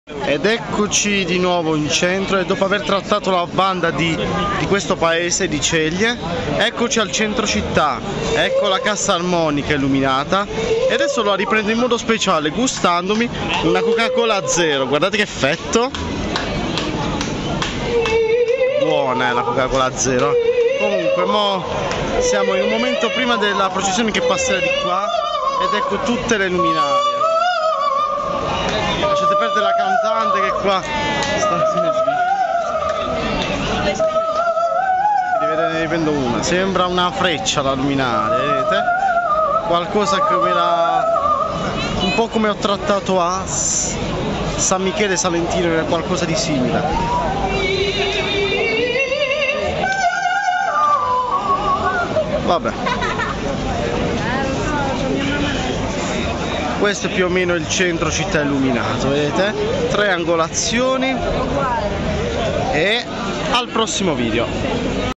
FESTA SAN ROCCO - ceglie messapica 17 agosto 2022 - (centro città illuminato)
E finiamo anche qui in bellezza una ripresa del centro città tra luminarie e cassa armonica